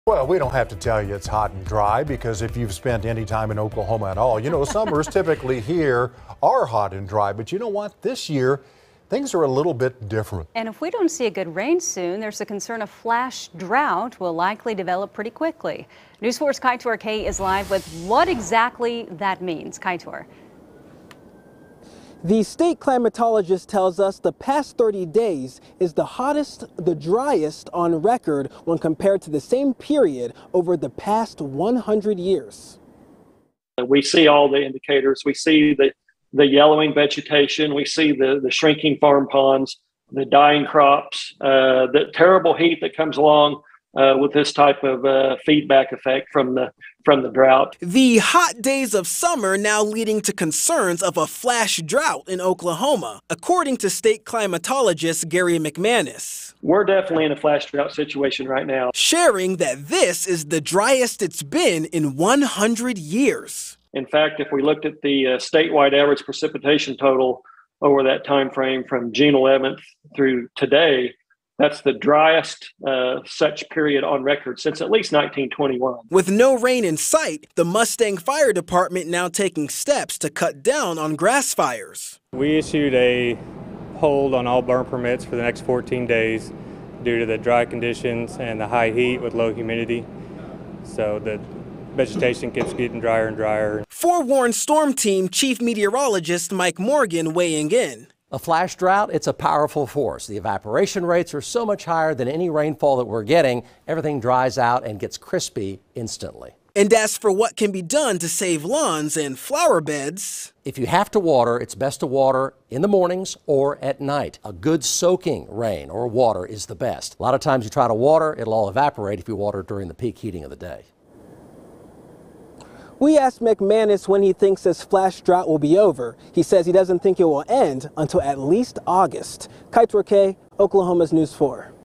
Flash drought concerns grow as Oklahoma dry weather continues
Oklahoma climate and weather experts are warning of flash drought conditions this summer